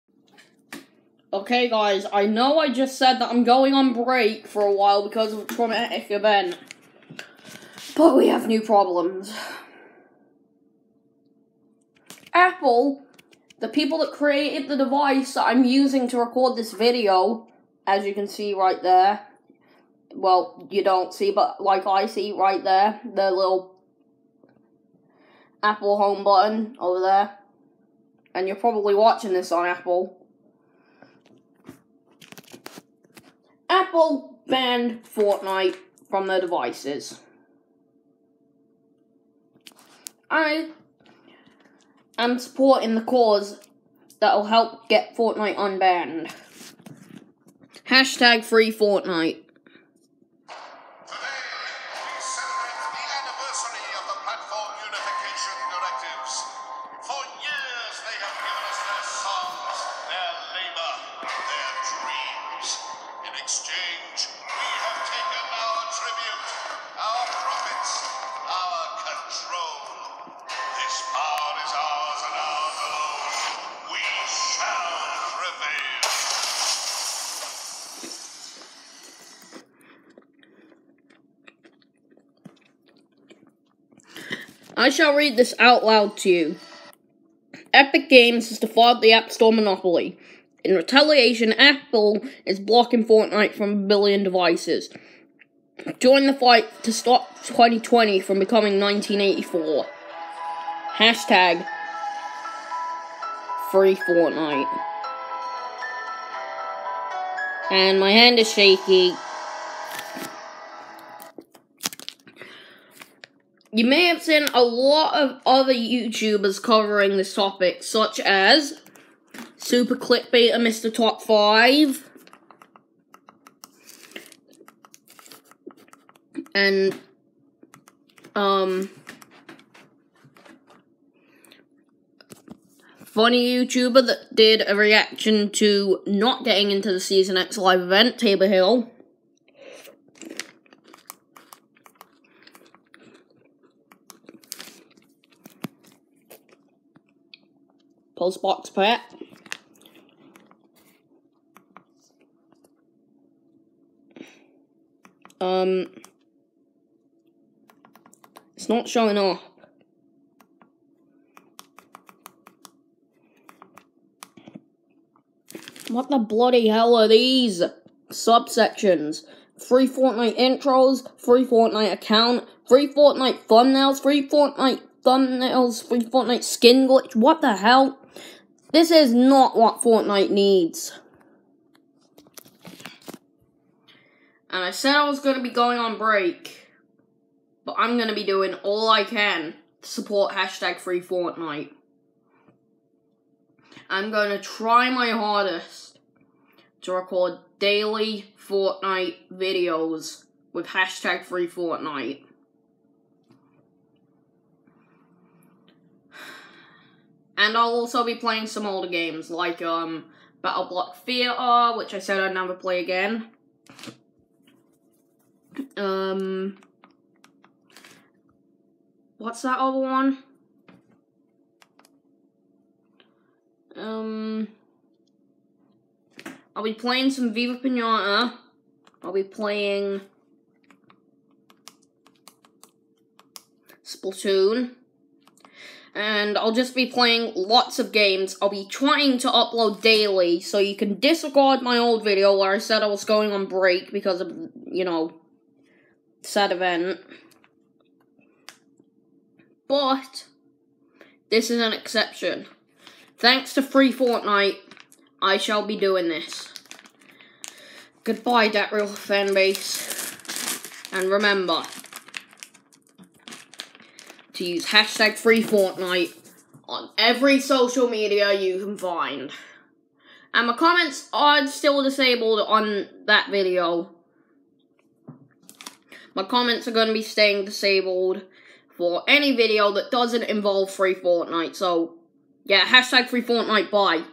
Forget that “break” I was on about, I’M BACK!
Epic Games have defied the App Store Monopoly. In retaliation, Apple, the people who I’ve TRUSTED FOR MANY YEARS, have decided to block Fortnite from billions and BILLIONS of devices. I’m not letting this slide. It’s time for the daily freefortnite upload spam. I’m joining the fight. Apple won’t make 2020 become 1984. And as for the whole Beaux situation? I’m a child-man, not a man-child. A dog isn’t enough to make me cry. See you on the battle bus, guys.